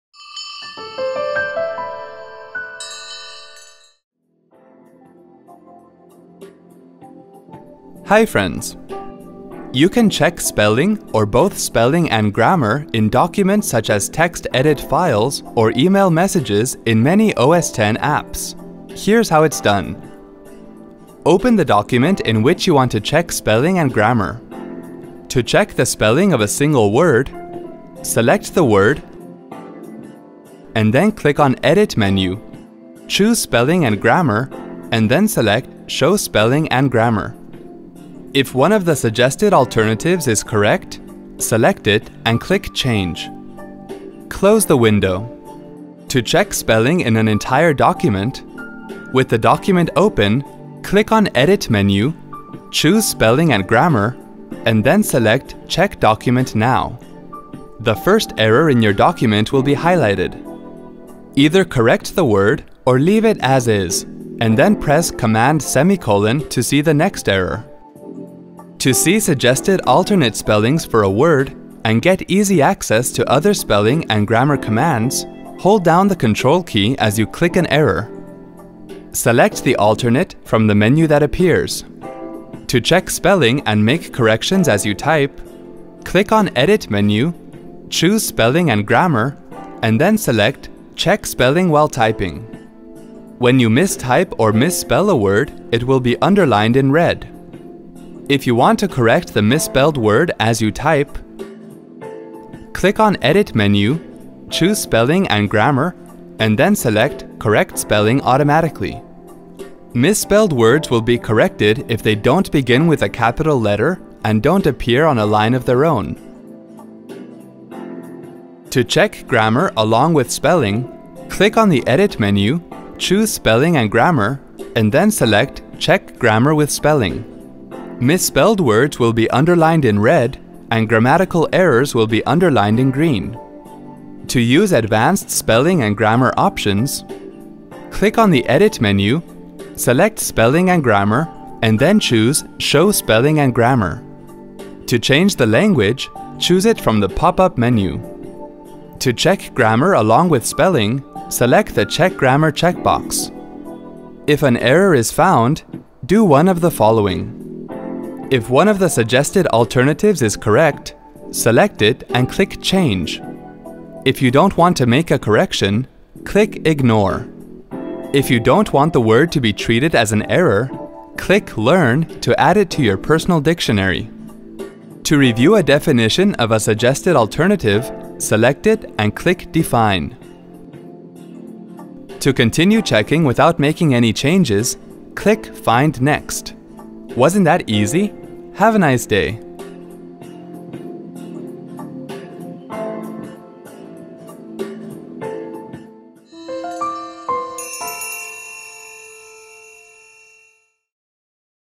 How to Check Spelling and Grammar in Mac® OS X™
You can check spelling, or both spelling and grammar, in documents such as TextEdit files or email messages in many of Mac® OS X apps. Want to check spelling and grammar in Mac® OS X™? Follow the easy steps shown in this video.